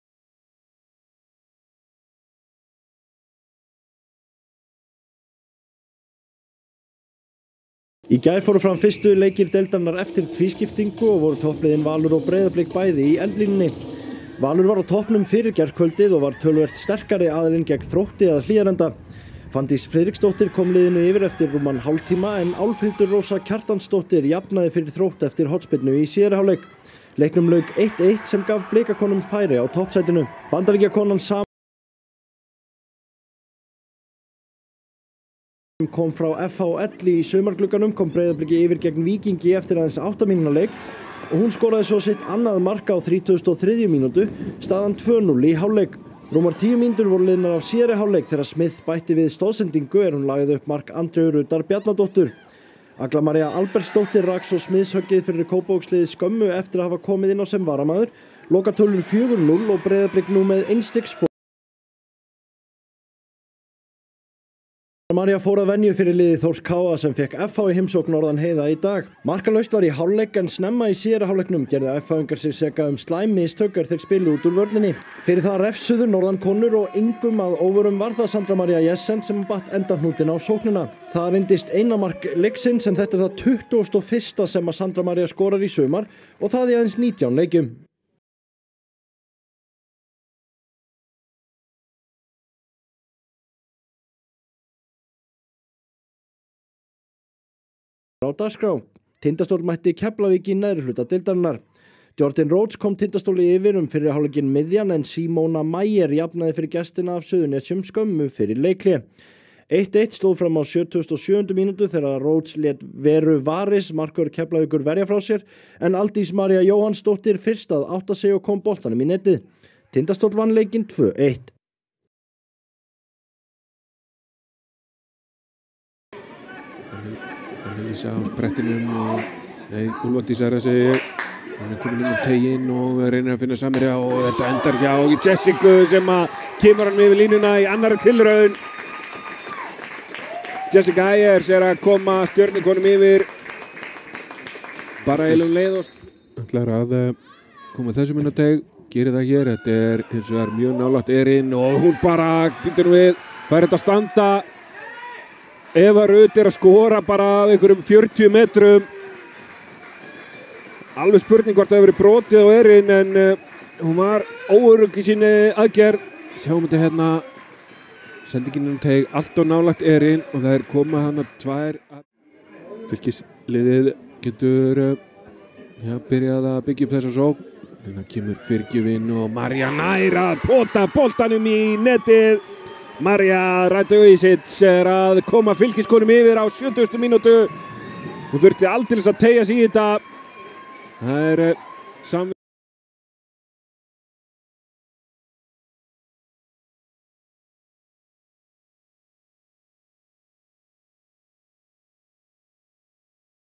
Besta Deildin 2024 (Championship / Releagation round day 1 women)
Here you can see all goals from Championship / Relegation round of Icelandic women's league.
5 games = 13 goals

Results:
CHAMPIONSHIP ROUND:
Valur - Thróttur 1:1
Breidablík - Víkingur 4:0
Thór/KA - FH Hafarfjordur 1:0

RELEGATION ROUND:
Tindastóll - Keflavík 2:1
Stjarnan - Fylkir 1:2